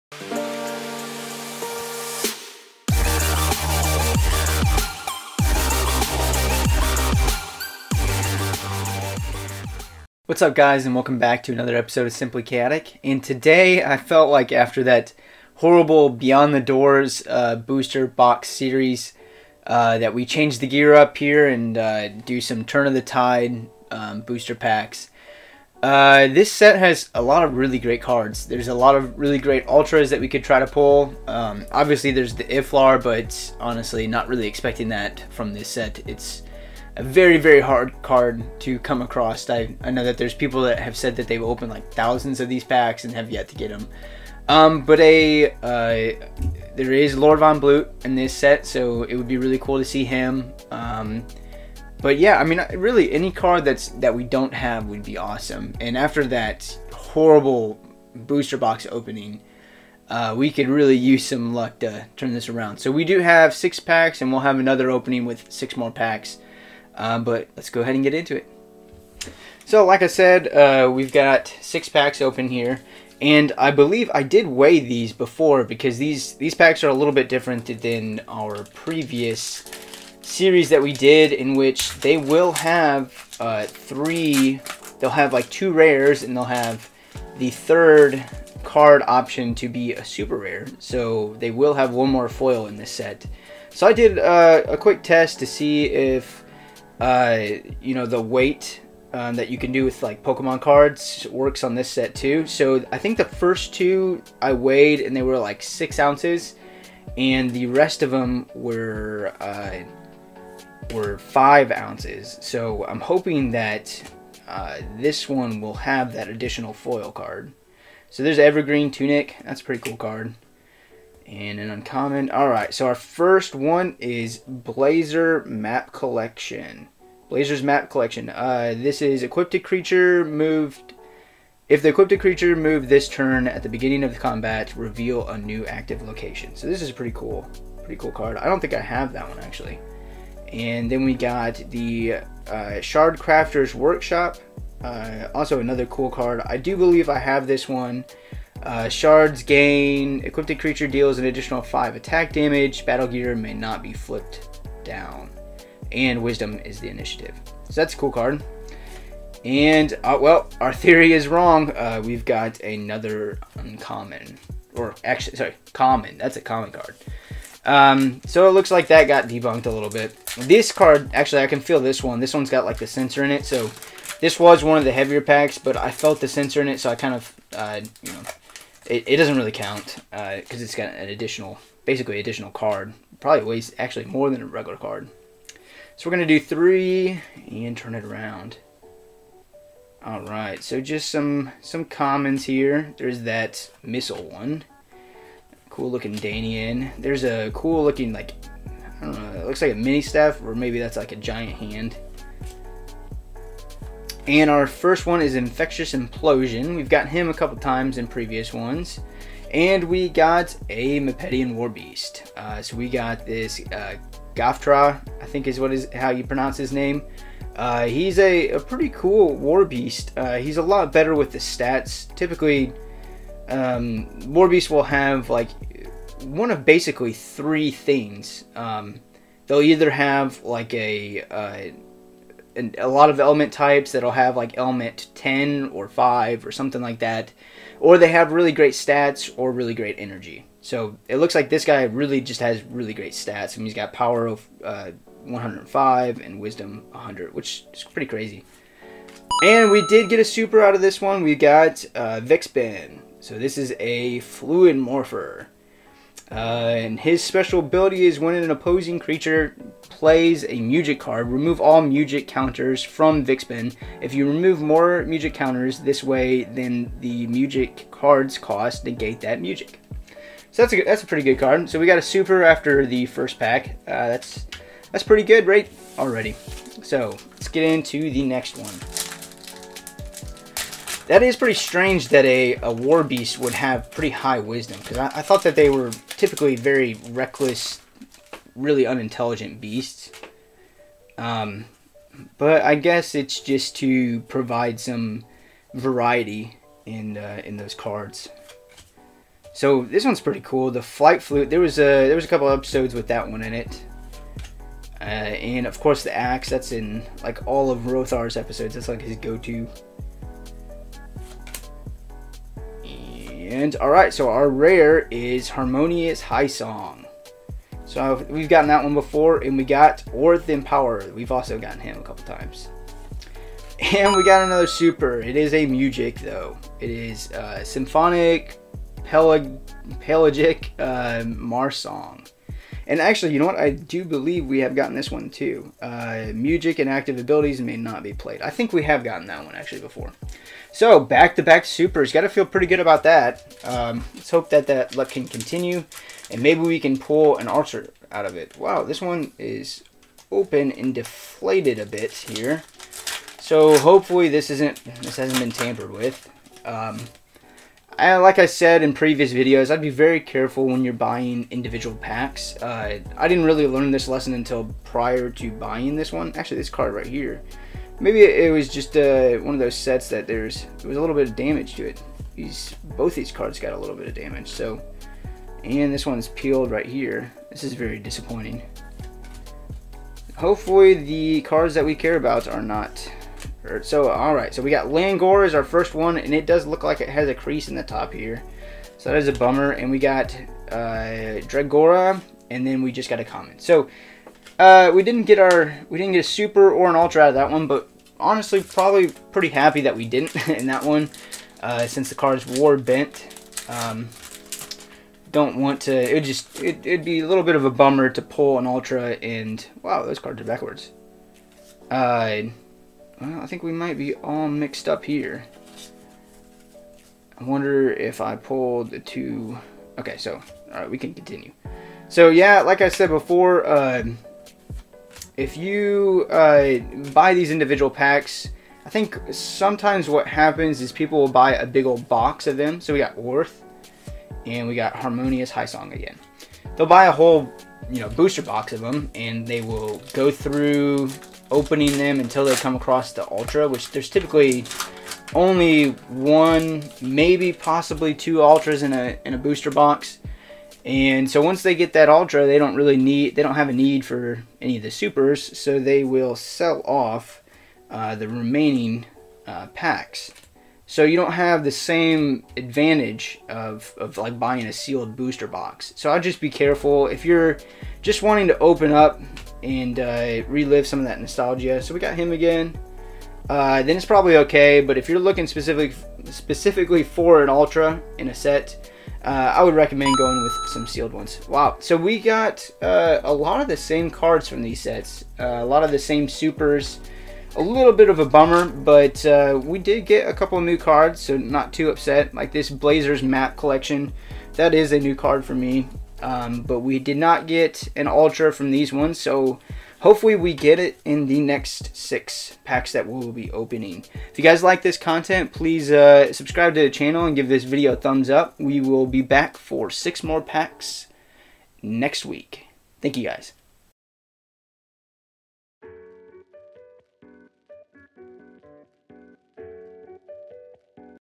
*Back with Turn of the Tides Boosters!*
Going to take a break from those Beyond the Doors booster boxes for a while... We are going to take some time and cool down with some Turn of the Tides Booster packs. Hoping for a good ultra!

If you are interested in more Chaotic TCG pack openings then this is the place for you!